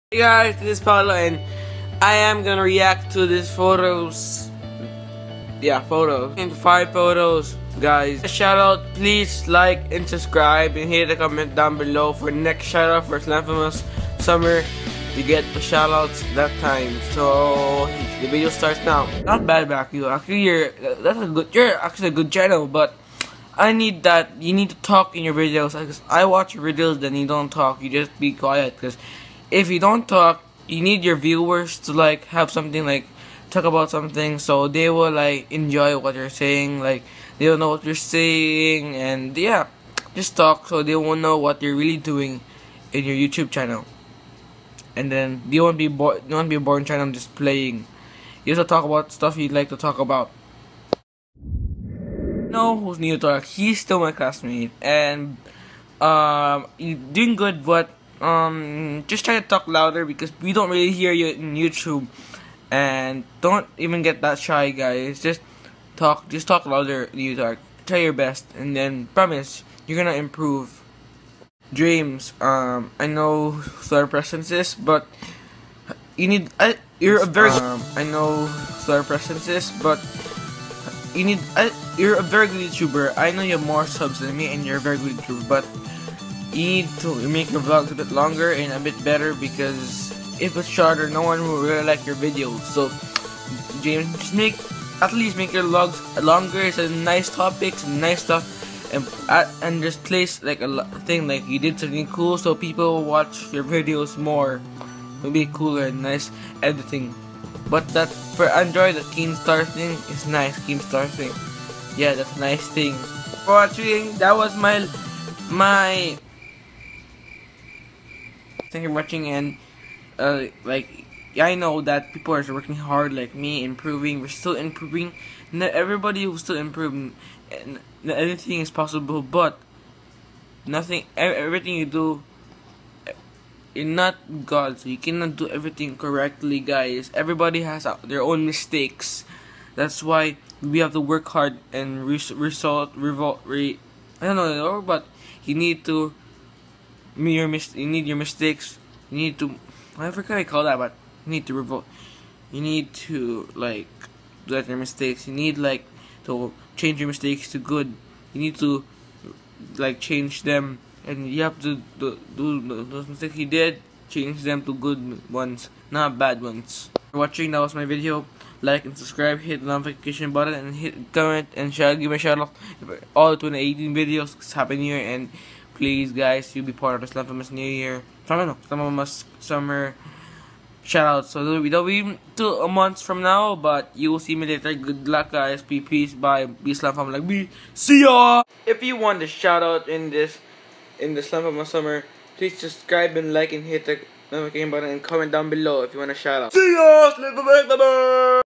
Reacting)with Paolo)#1
And guys my topic for that was my three top best slamfamers and they will be part of the slamfamus summer.
Don't forget if you have a iOS device downloaded iMovie for editing.